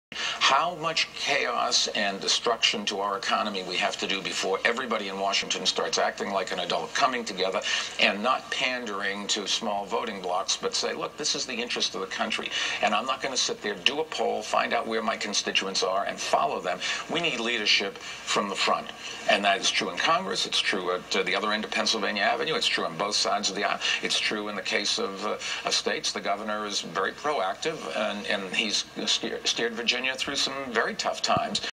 Mayor Bloomberg says president needs to Man up
Well, he was careful with his choice of words, actually, but Mike included congress and the other side of Pennsylvania Avenue in the list of places that aren't coming thru for America on facing up to budget issues.